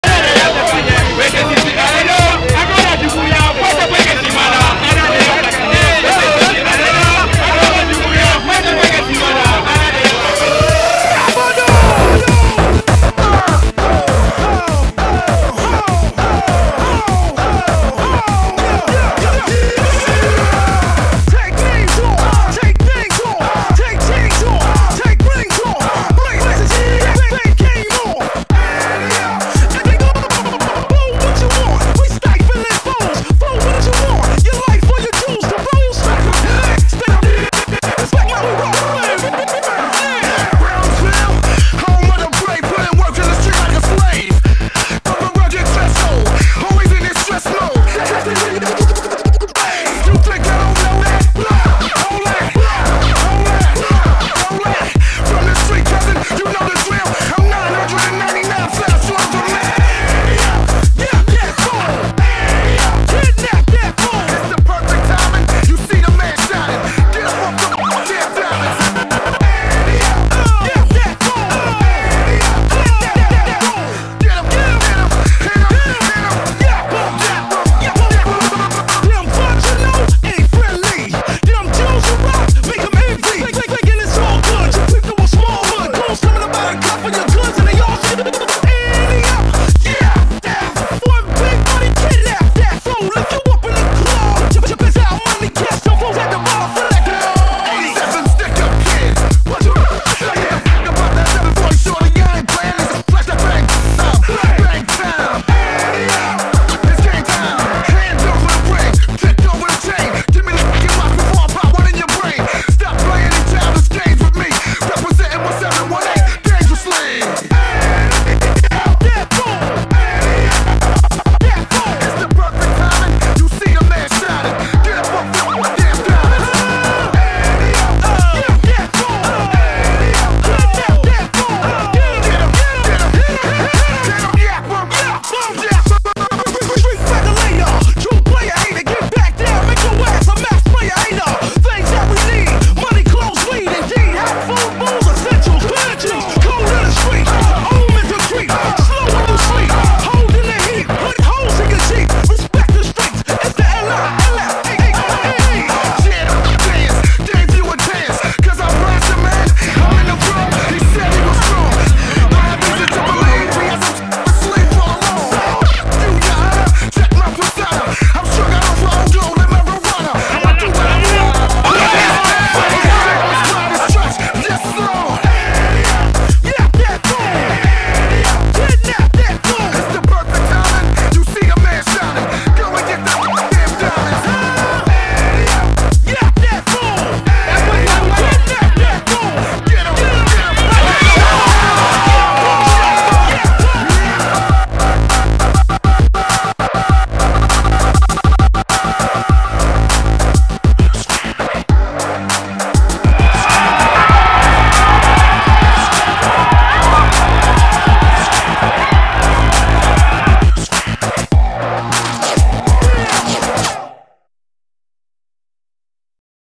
patin cross
LE GROUP PATINC ROSS de faladje FAIT LE CASCADE DE L'ANNEE..ON A LE RECOR DU MALI EN CASCADE